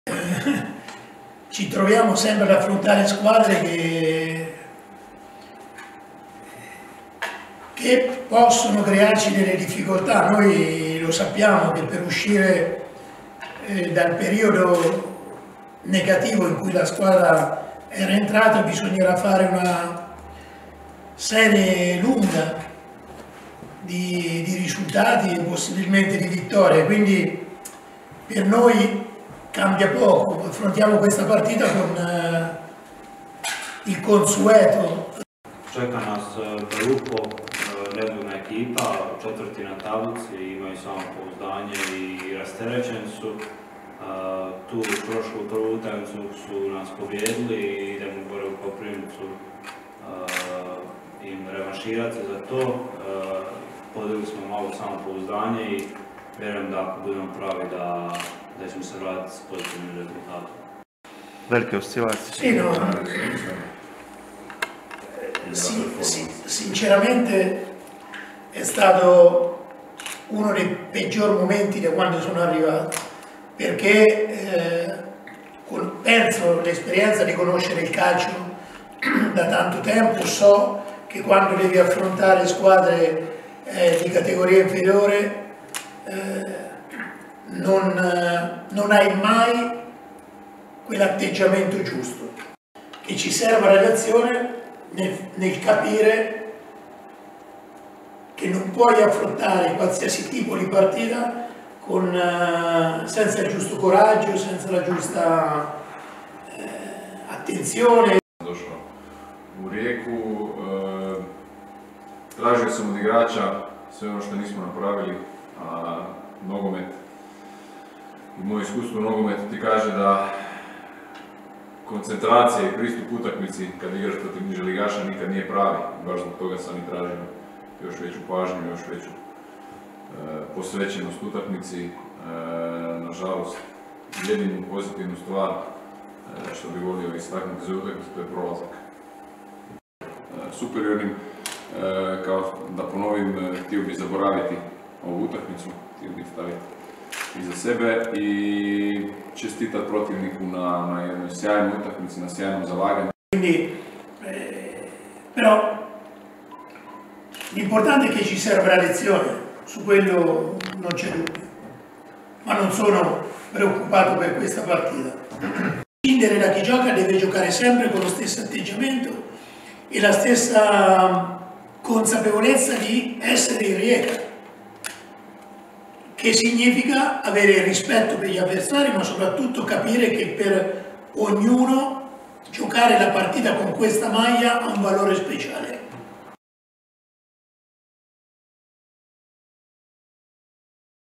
Serse Cosmi i Ivan Smolčić najavili su gostovanje Rijeke kod Slaven Belupa
Trener HNK Rijeka Serse Cosmi i igrač Ivan Smolčić održali su konferenciju za medije u press centru stadiona HNK Rijeka na Rujevici uoči susreta 14. kola SuperSport HNL-a između Slaven Belupa i Rijeke.